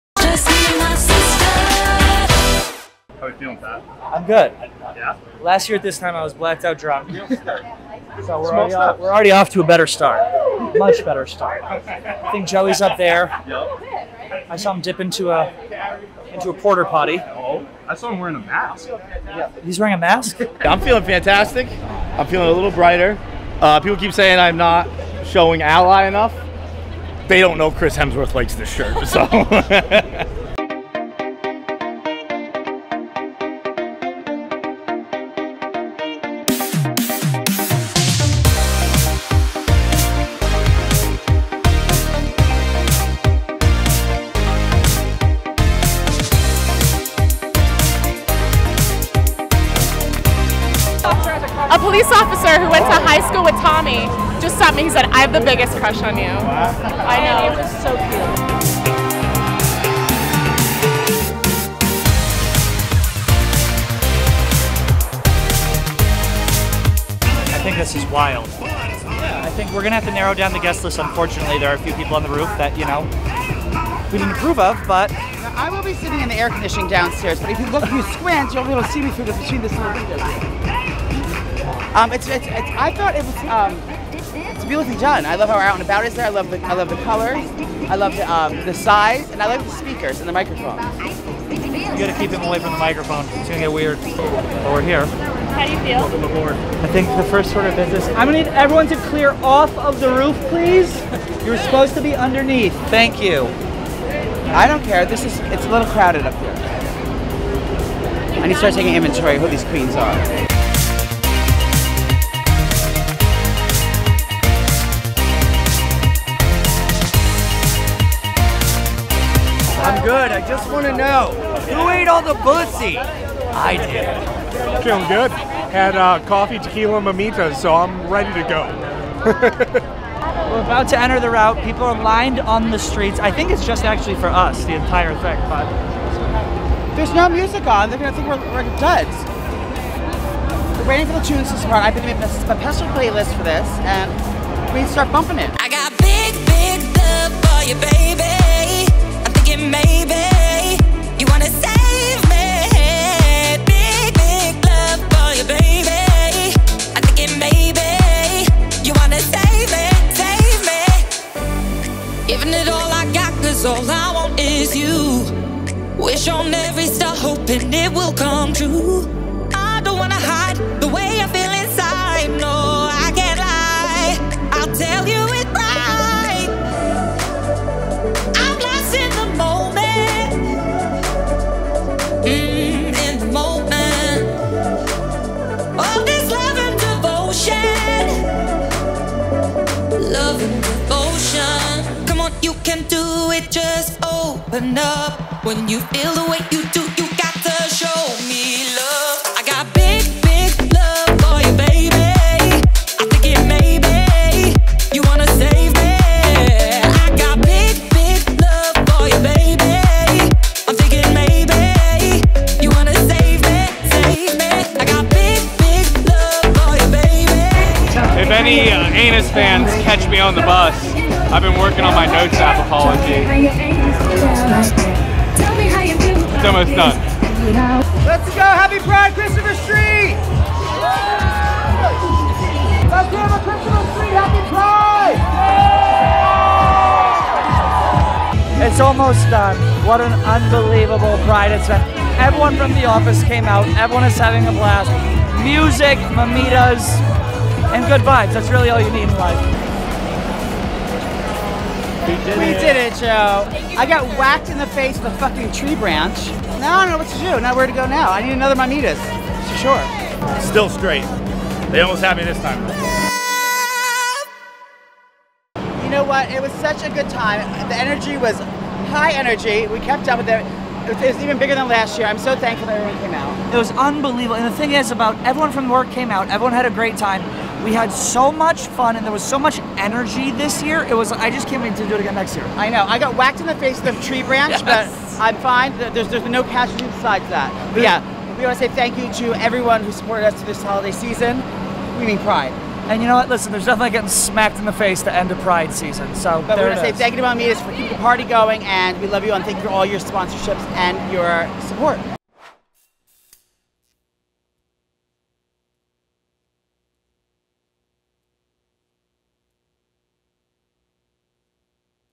Barstool Sports Attends the NYC Pride Parade
Join us behind the scenes with Barstool at the New York City Pride Parade.